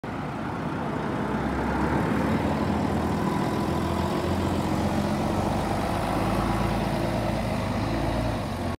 Transit Systems 1275 Volvo B12RLE Bustech VST 804 To Liverpool
Built 2007
Route 804 to Liverpool
Time 8:14am 1 minute early
Fleet number 1275
Type Volvo B12RLE Bustech VST